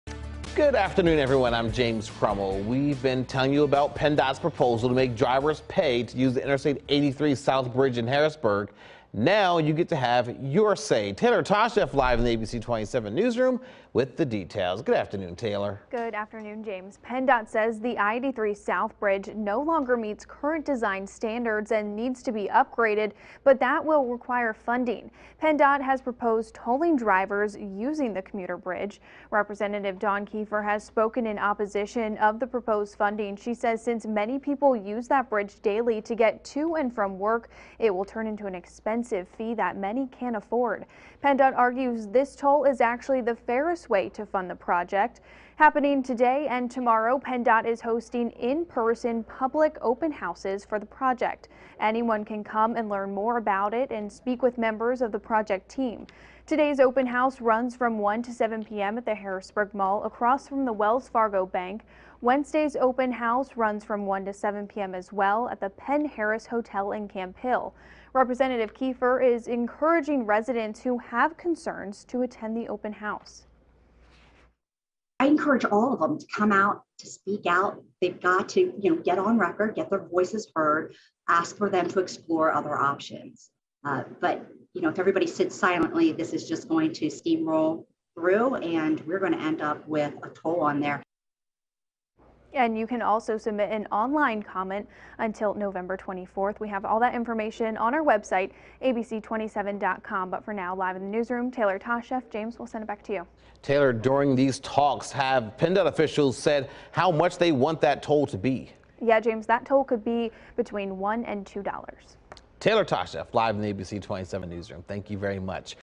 PennDOT invites public to comment on I-83 South Bridge reconstruction and toll project
PennDOT is inviting the public to comment on the I-83 South Bridge replacement project. PennDOT says the bridge no longer meets current design standards and needs to be upgraded, but that will require funding.